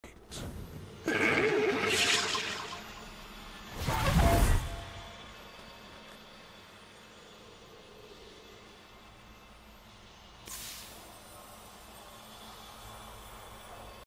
Thank you bungie XD
Having a bit of fun with the new sword perk and an afk player